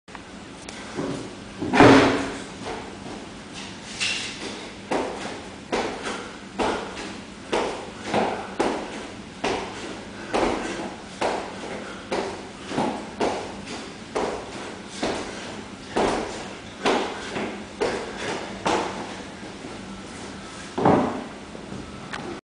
Jumping 2
Short, physical studio exploration.  Jumping as high as I can.